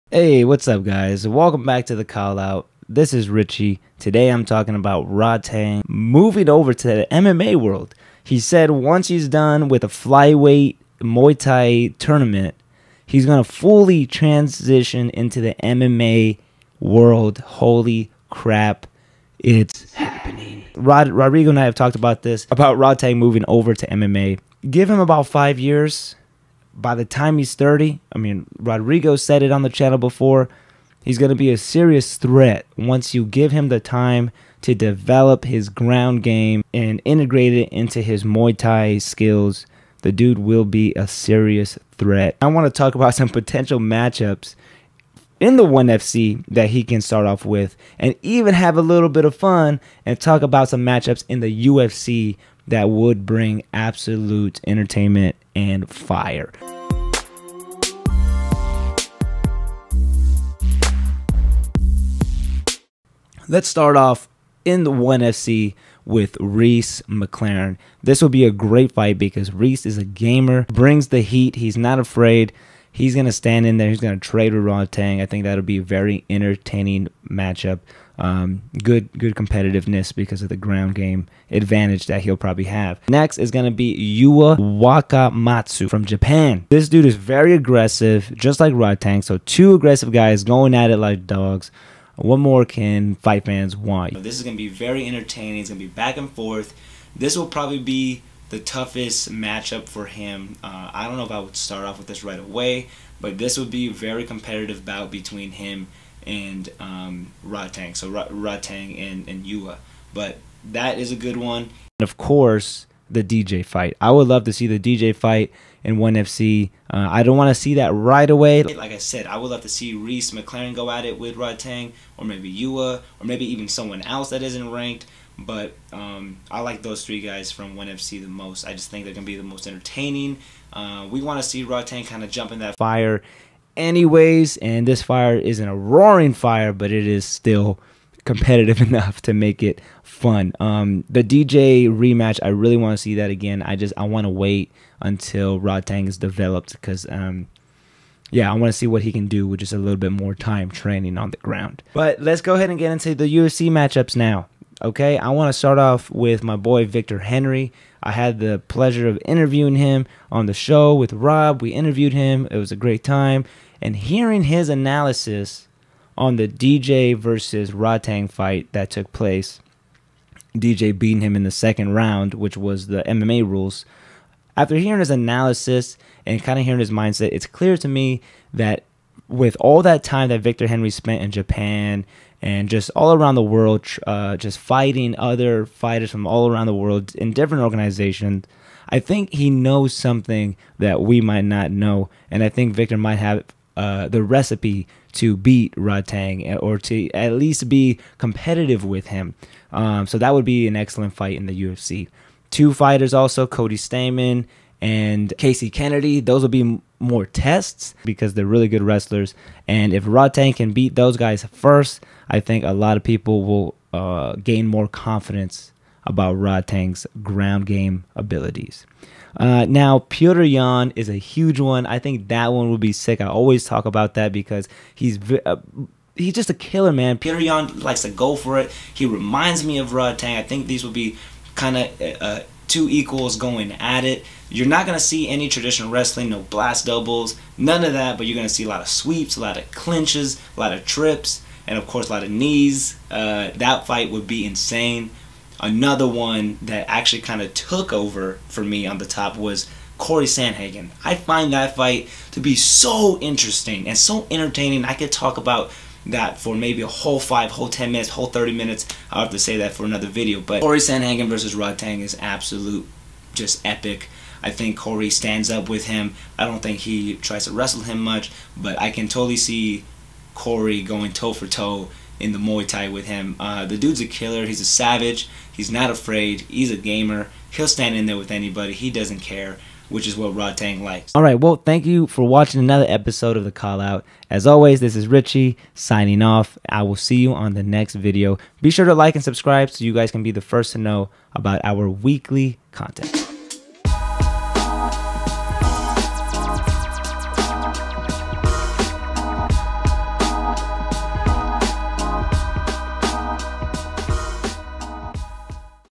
Rodtang to Fully Transistion into MMA?! Let’s talk potential opponents 😎 💥
Rodtang plans to fully transition into mma after Fyweight Muay Thai World Grand Prix. Who are some opponents you hope to watch Rodtang face in MMA?